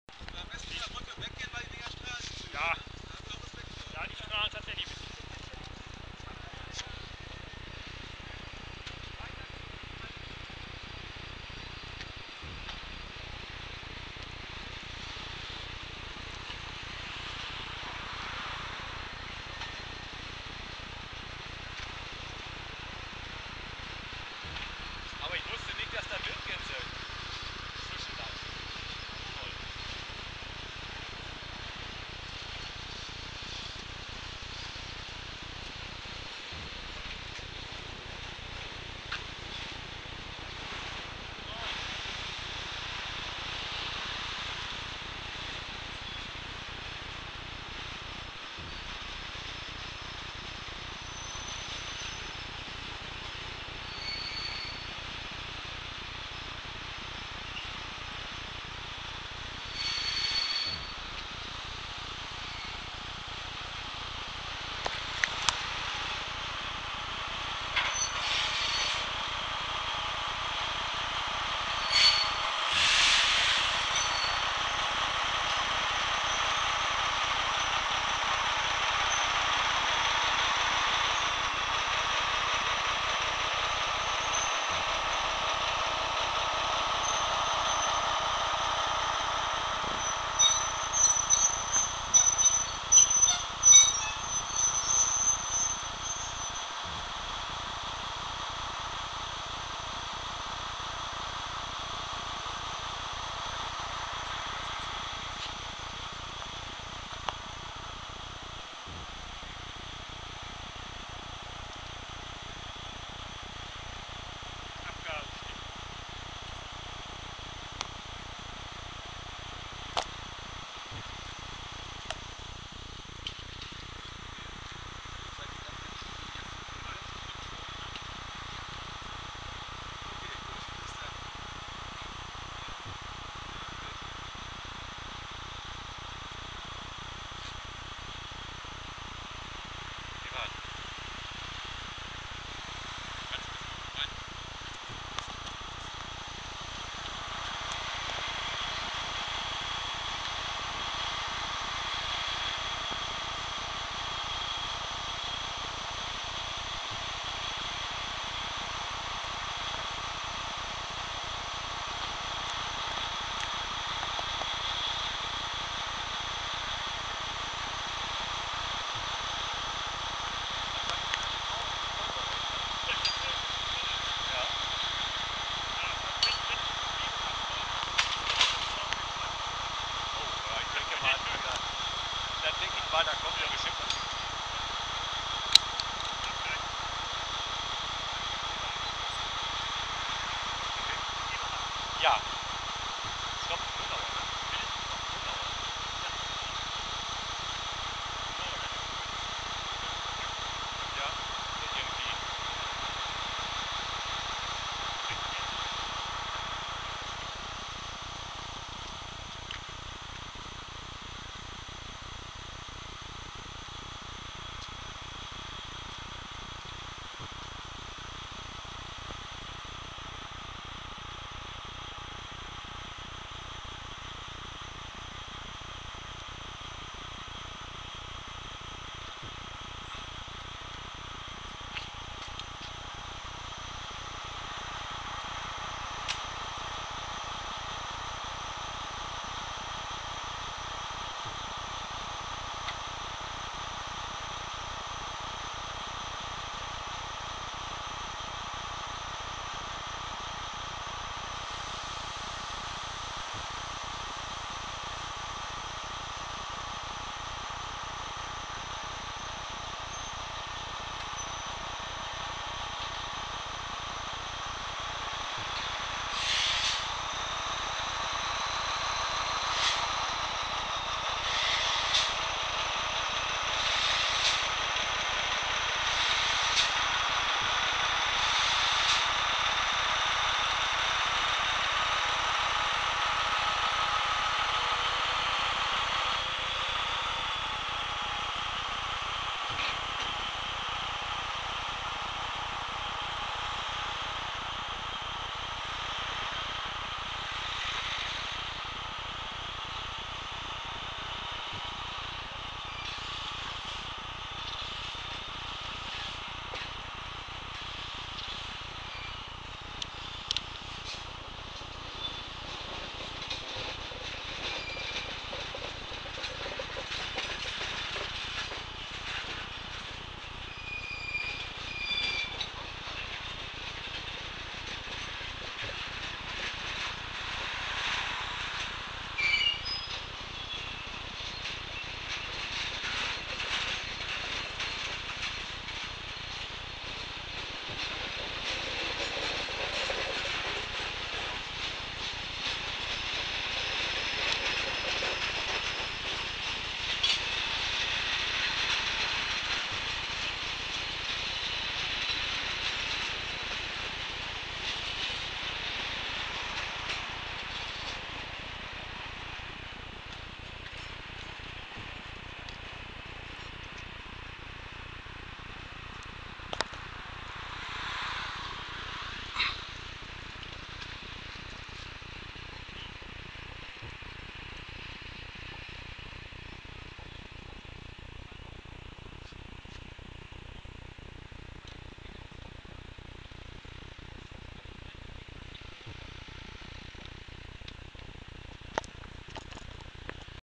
22.10.2012 Gefährlicher Urantransport von Urenco Gronau nach Frankreich
Am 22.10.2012 schon wieder: Von der Uranfabrik in Gronau/Westf. setzt sich still und heimlich ein gefährlicher Urantransport in Bewegung, vermutlich nach Frankreich.